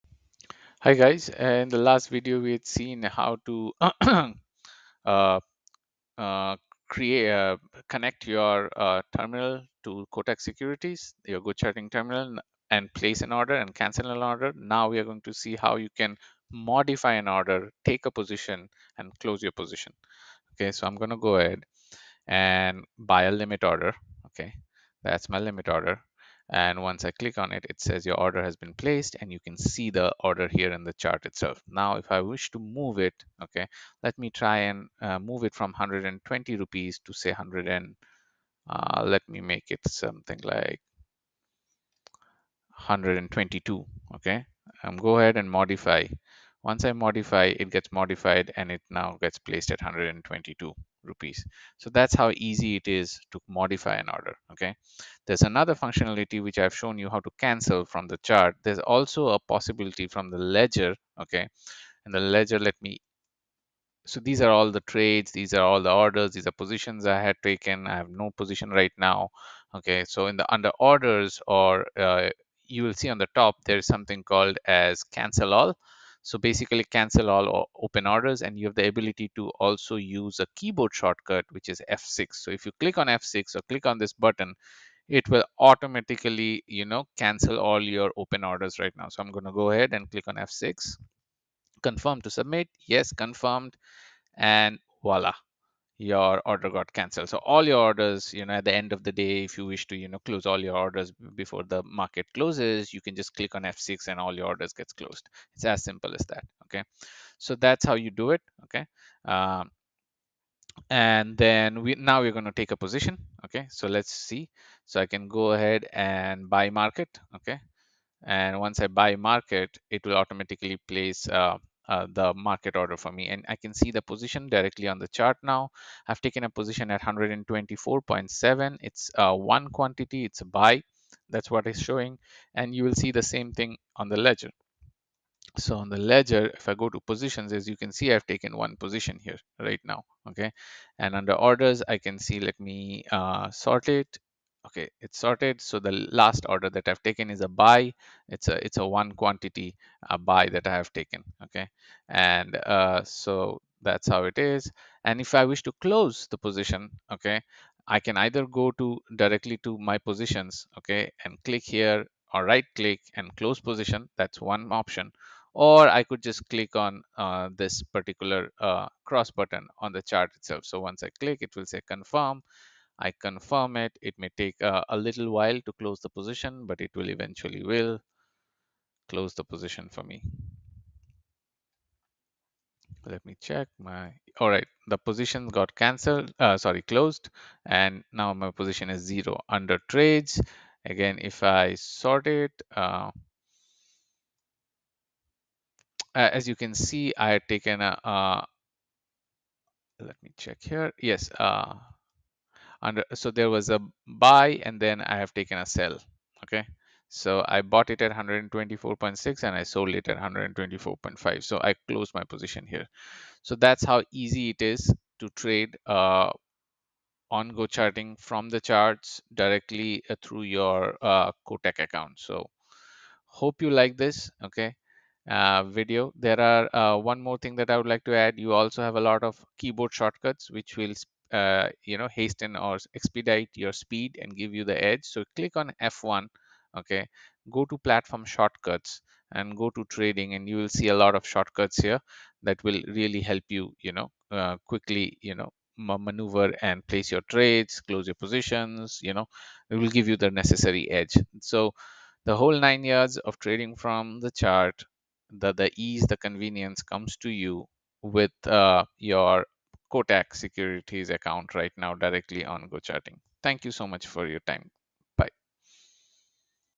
Live Trading from GoCharting Charts using Kotak Securities (2/2)
The video explains how to start trading on GoCharting using your Kotak Securities account.  Seamless and blazing fast live trading directly from GoCharting Charts.

In this video, we demonstrate how you can place an order from the chart, modify the order, cancel the order, track your positions and close your positions, key board shortcuts and much more

Please also watch the prequel to this video that explains how to connect GoCharting to Kotak Securities account and place an order from the chart